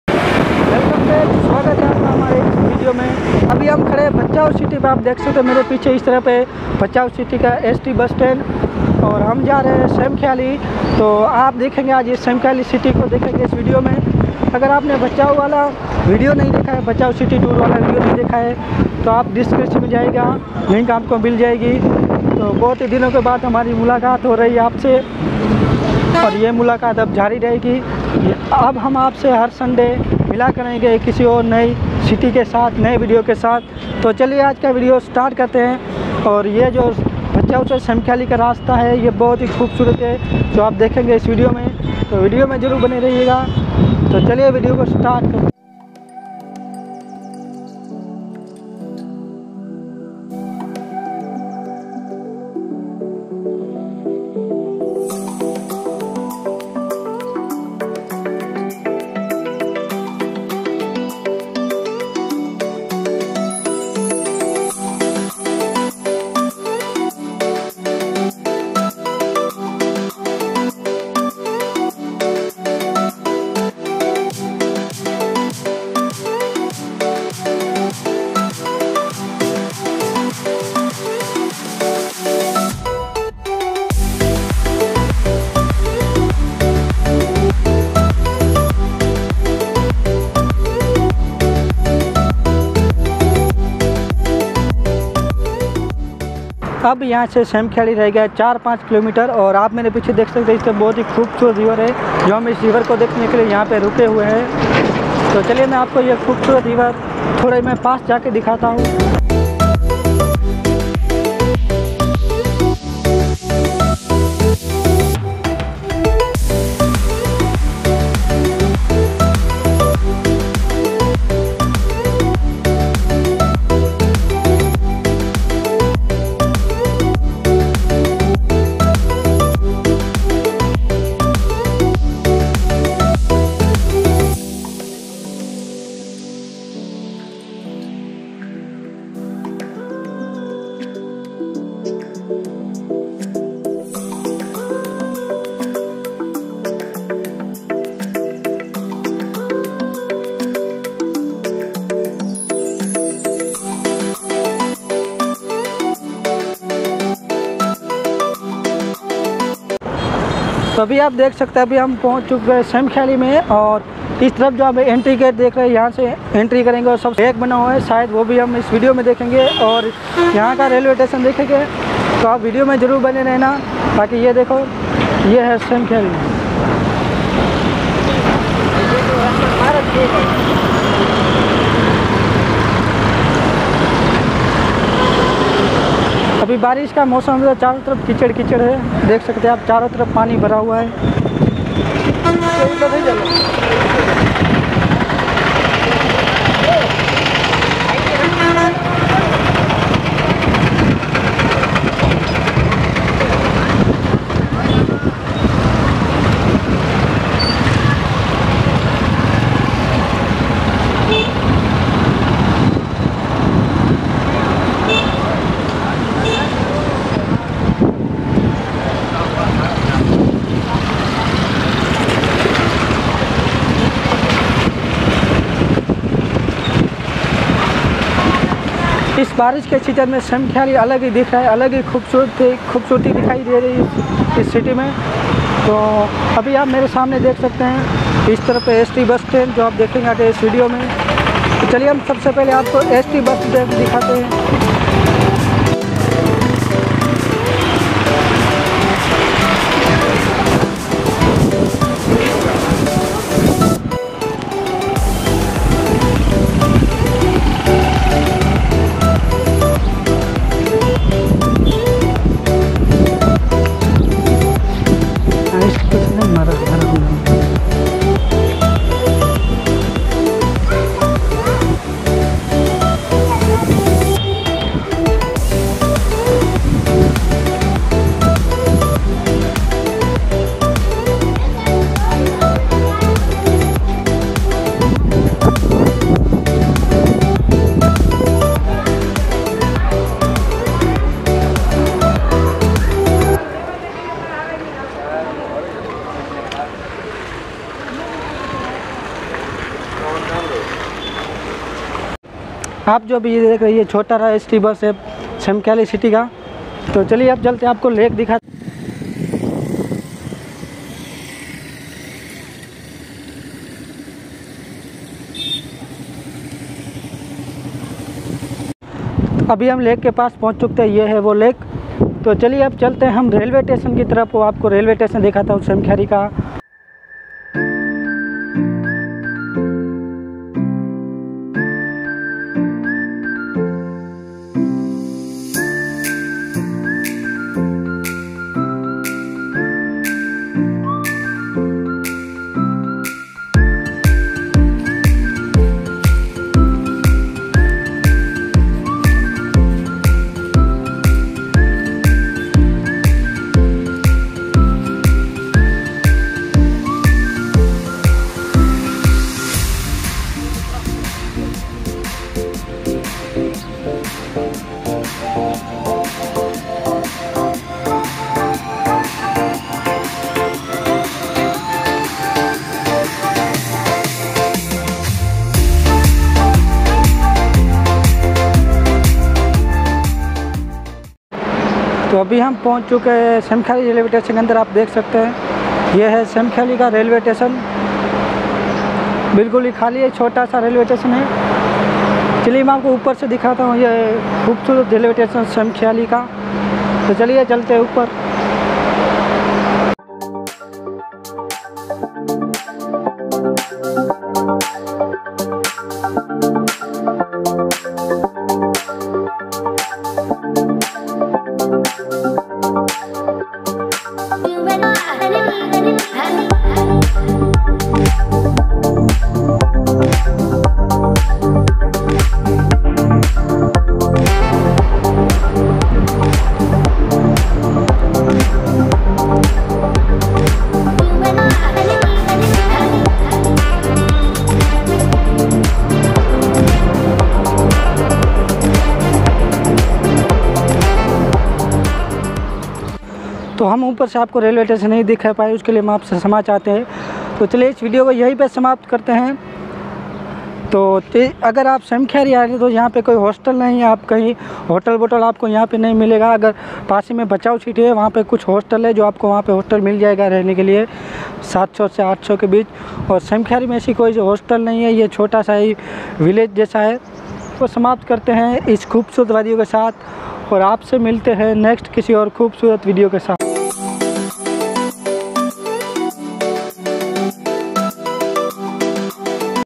samakhyari kutch | Bhachau to samakhyari | samakhyari City tour / st bus station and railway station
samakhyali City tour

wait for the next video gays